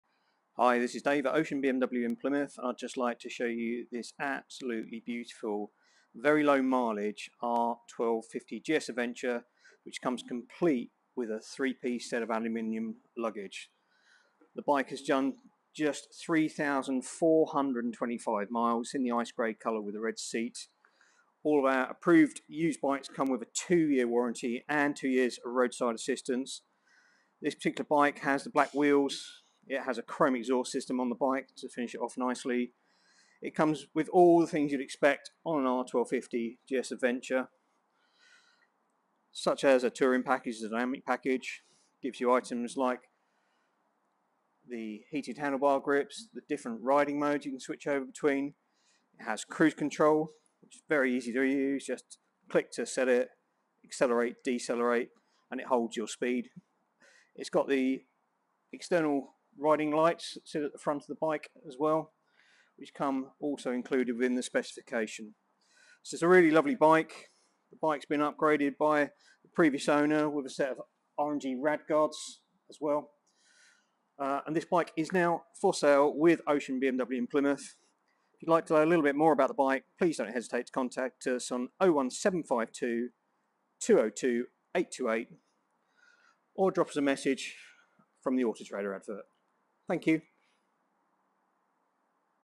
R1250GS Adventure with 3 Piece Luggage
This beautiful approved used bike is for sale at Ocean BMW Plymouth.